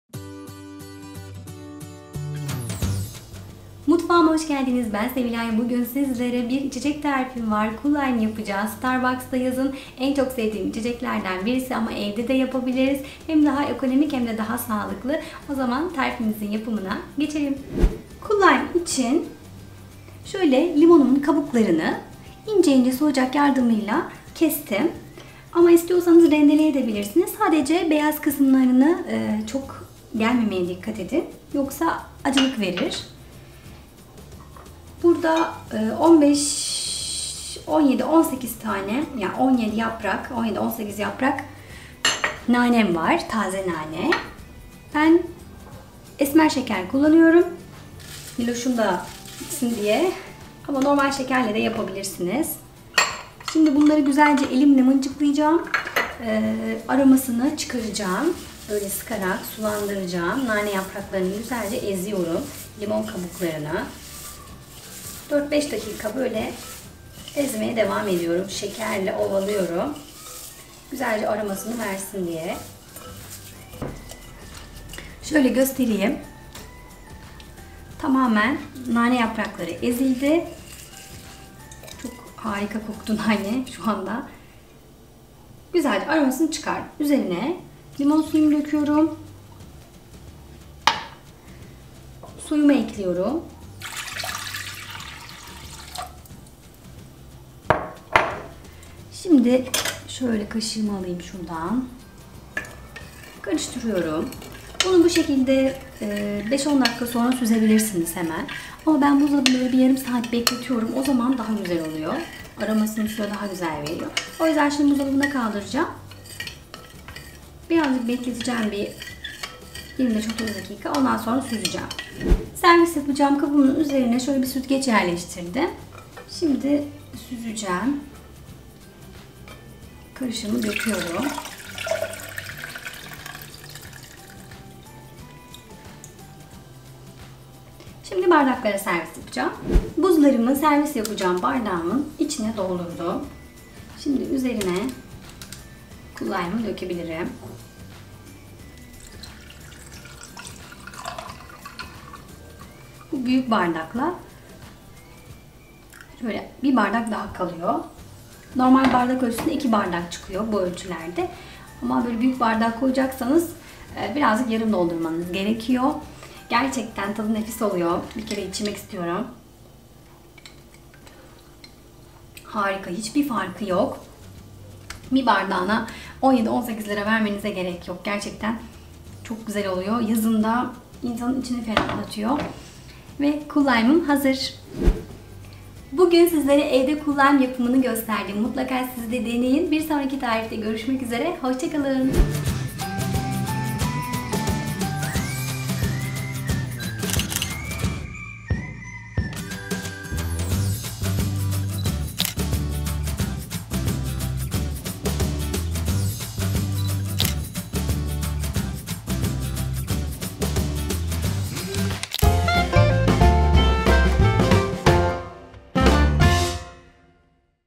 Starbucks Cool Lime Tarifi | Starbucks Cool Lime Nasıl Yapılır?
Starbucks Cool Lime Tarifi | MALZEMELER
1 adet limon suyu
1 adet limon kabuğu
15 20 yaprak nane
2-3 çorba kaşığı toz şeker
2 su bardağı su

Herkes Yemek Yapabilir! | Sevilay Bağ'ın sunumuyla birbirinden özel ve pratik videolu yemek tarifleri Sevilay'ın Mutfağı YouTube kanalımız ve sevilayinmutfagi.com 'da...